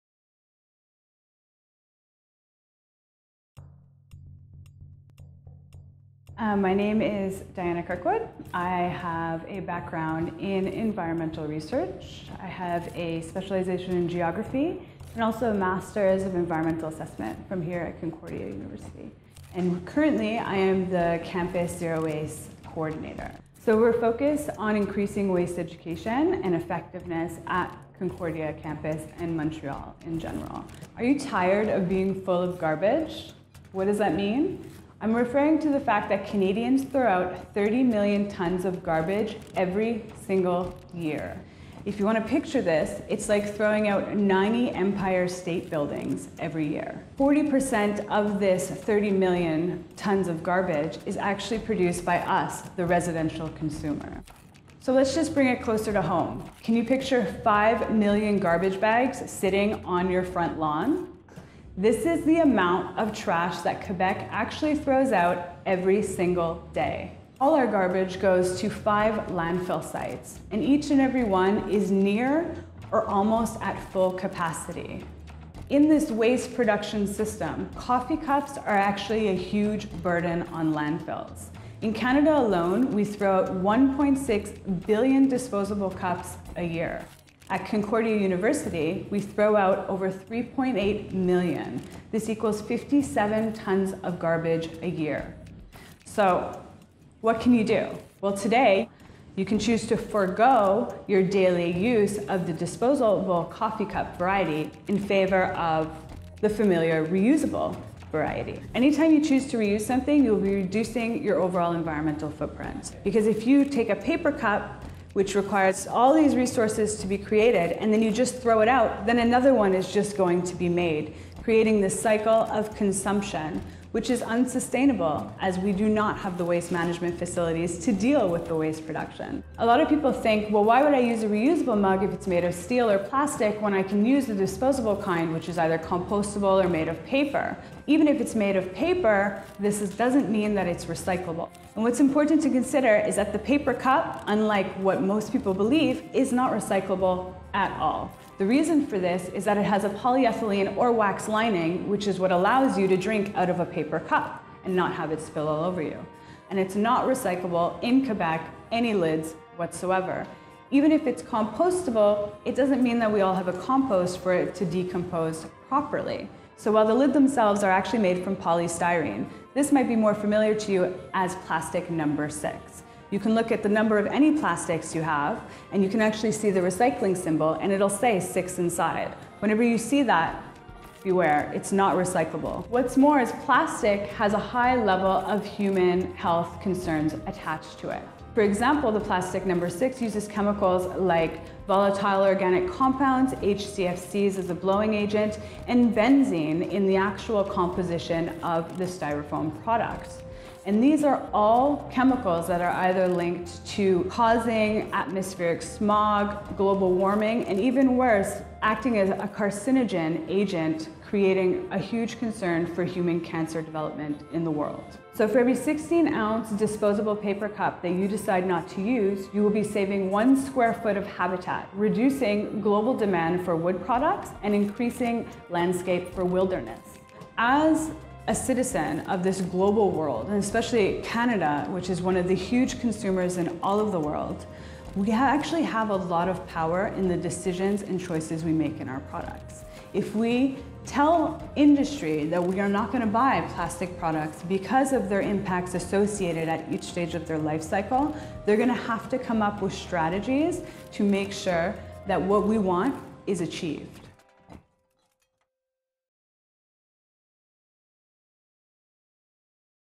Footprint of a coffee cup
Are your tired of being full of garbage. Join Diana Kirkwood the Zero Waste Campus Coordinator at Sustainable Concordia, as she walks through the reals cost of disposable coffee cups and offers tips for reducing your environmental footprint.